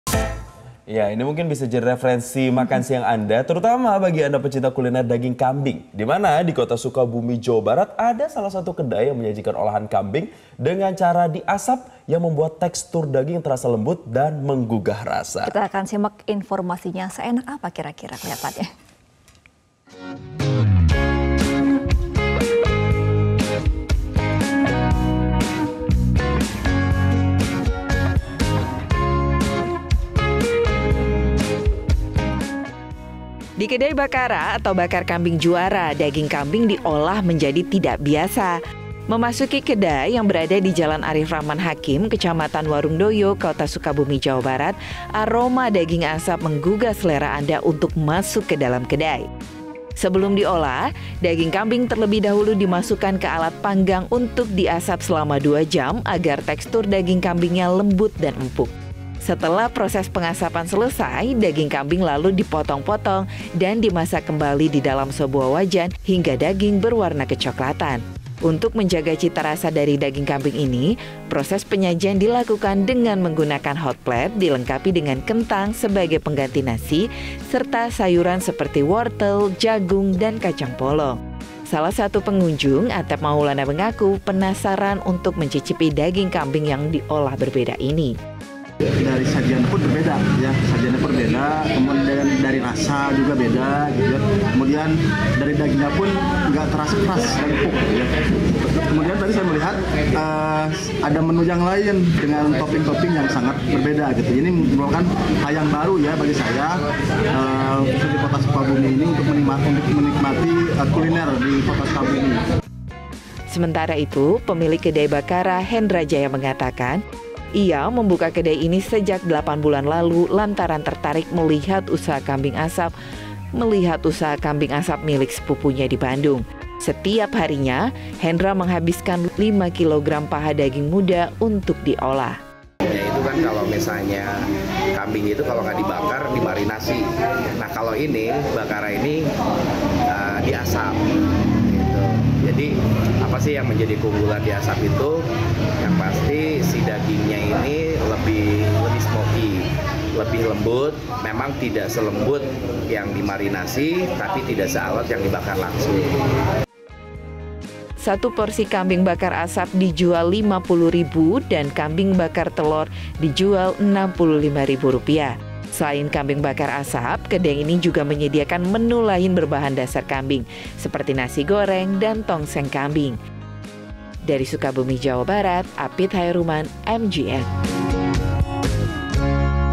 Sajian Daging Kambing Asap Menggugah Selera
MetroTV, Sajian ini bisa menjadikan makan siang anda, bagi kalian yang mencintai kuliner daging kambing dimana kota Sukabumi Jawa Barat yang menjadikan salah satu kedai dengan olahan Kambing dengan cara di asap agar membuat tekstur daring menjadi lembut dan menggugah rasa